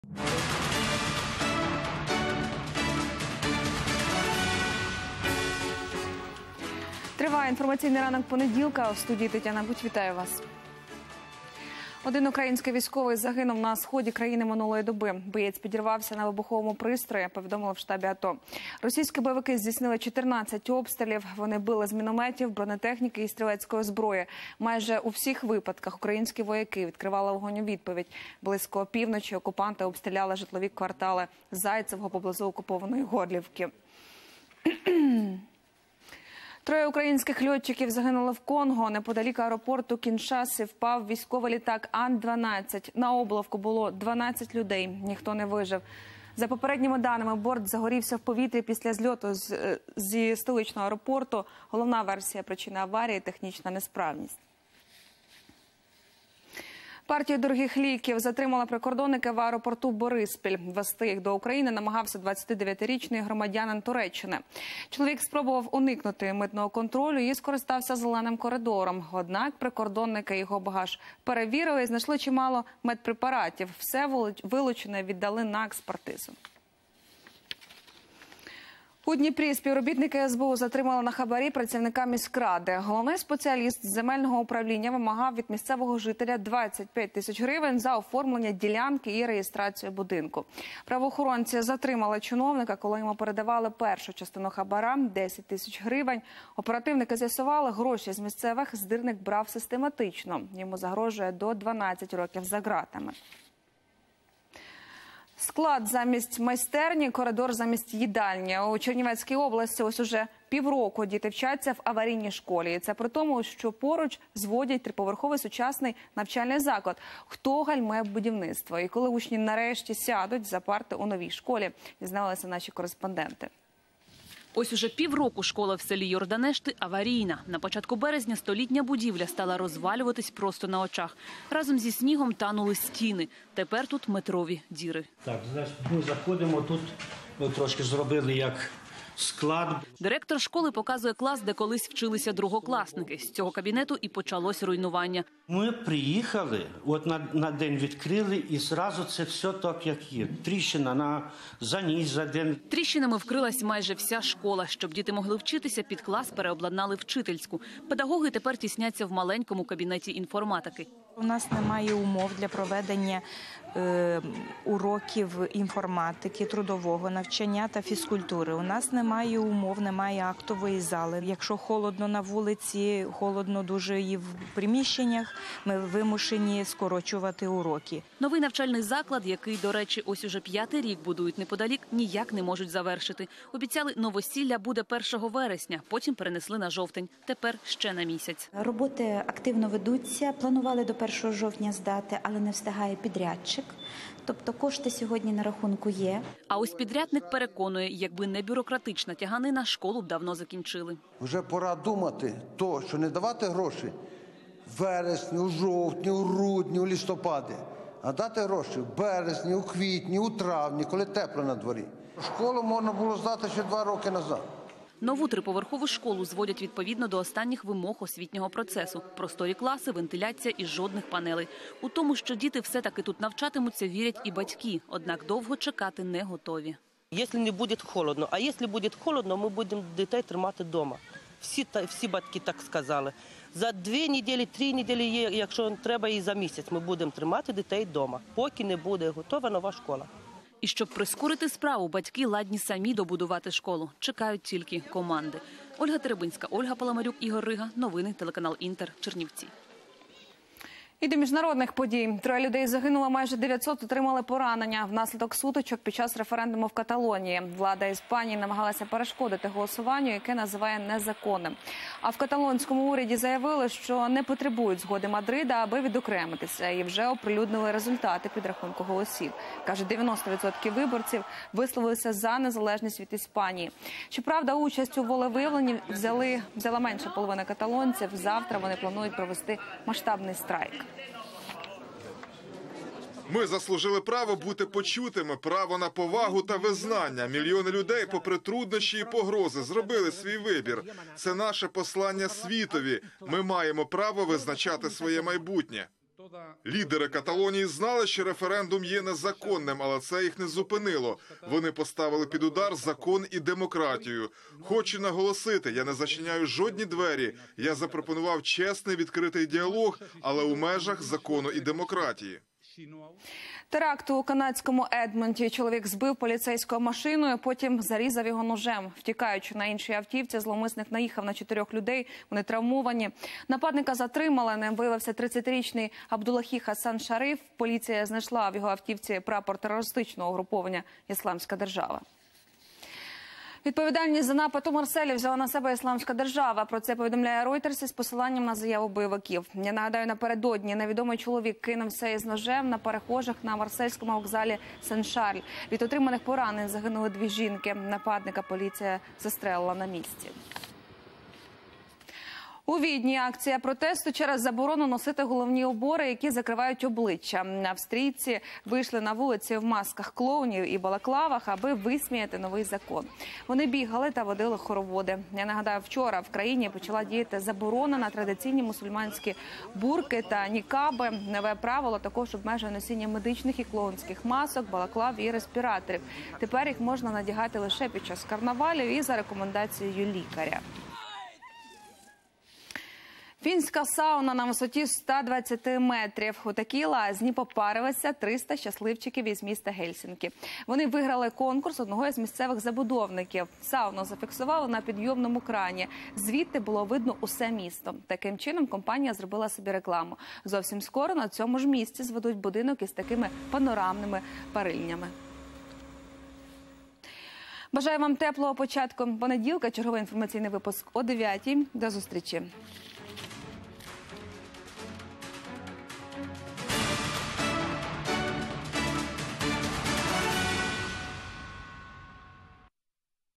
Новости 8:00, выпуск за 2 октября 2017 года
Головні події на цю годину: На Донеччині на вибухівці підірвався армієць - штаб АТО; В Конго у авіакатастрофі загинули українські пілоти; Митники аеропорту "Бориспіль" затримали партію контрабандних ліків (відео); У Дніпрі місцевий чиновник вимагав хабар за реєстрацію нерухомості (відео); У Чернівецькій області діти півроку навчаються у аварійній школі; Влада Іспанії не визнає результати референдуму у Каталонії; У Канаді зловмисник автівкою понівечив людей; Відповідальність за напад у Марселі взяла на себе "Ісламська держава" - ЗМІ; У Австрії масово протестують проти заборони паранджі; У Фінляндії 300 щасливчиків попарилися у лазні на висоті 120 метрів (відео).